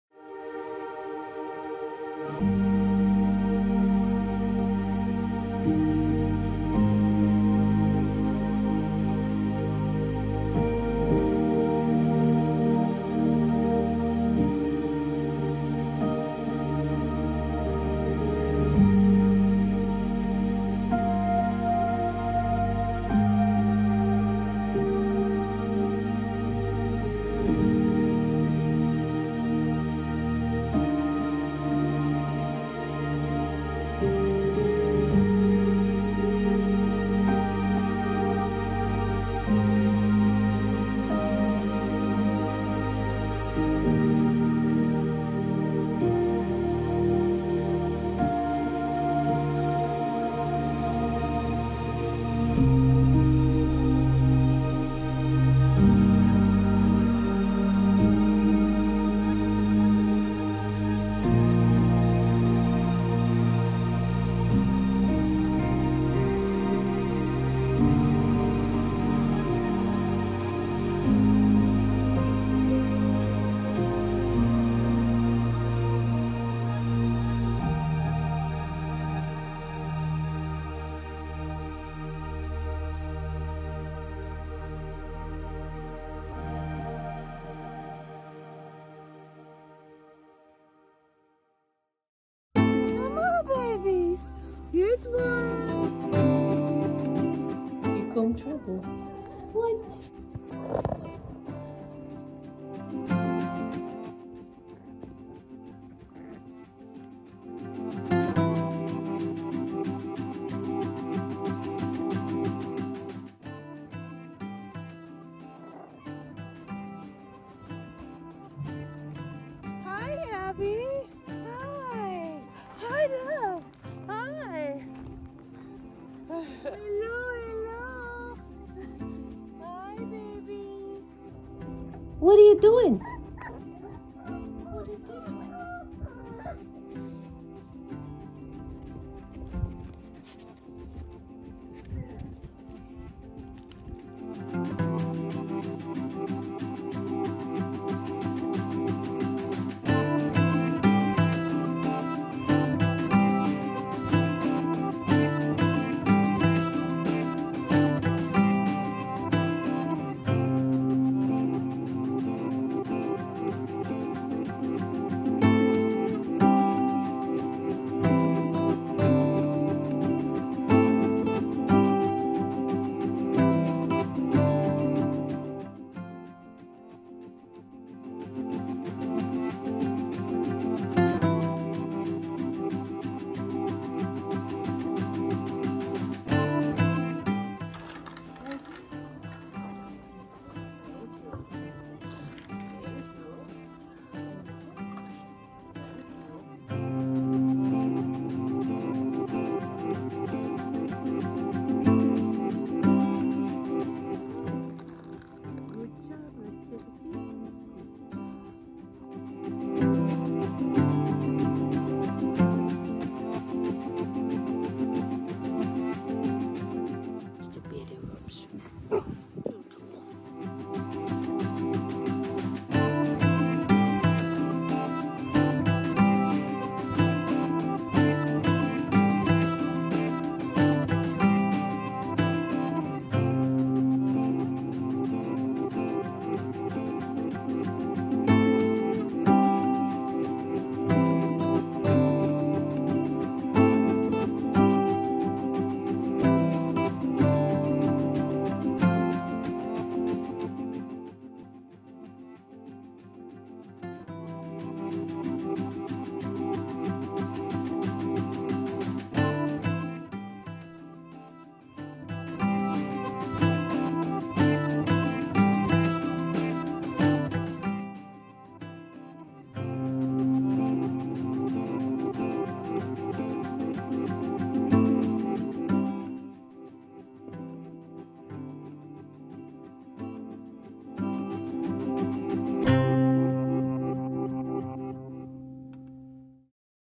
Old School Dogs Rescue: Leto and her pups.
Old School Dogs rescues an Aussie Cattle Dog and her nine pups.